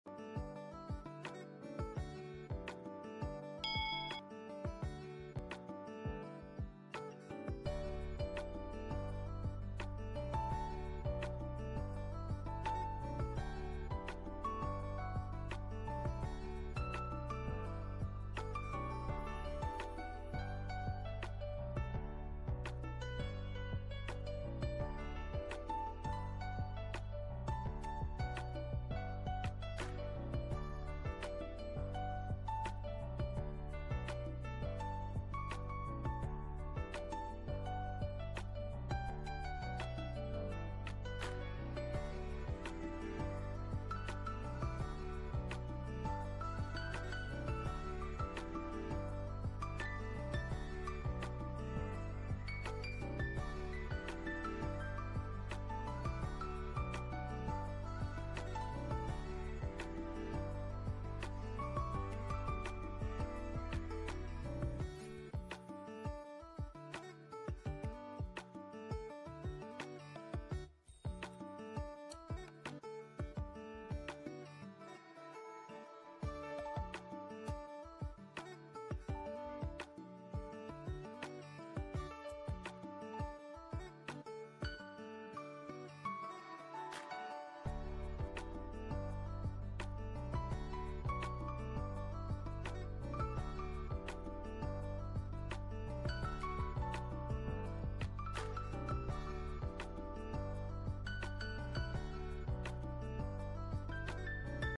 How to grow hair black and thick within a week using bhingraj/మీ జుట్టు నల్లగా,పొడువుగా రావాలి అంటే
Bhringraj

Hibiscus flowers

Hibiscus leaves

Curry leaves

Neem leaves

Soaked fenugreek seeds

Apply weekly twice u can get good results